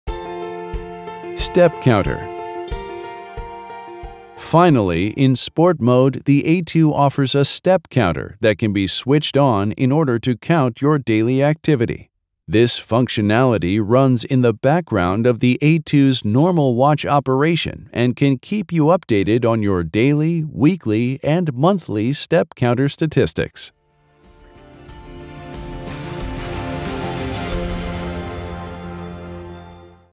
Aladin A2 Dive Computer Surface Features - Step Counter
The Aladin A2 dive computer in sport mode includes a step counter to monitor your daily activity. Aladin A2 can keep you updated on your step counter statistics on a daily, weekly or monthly basis.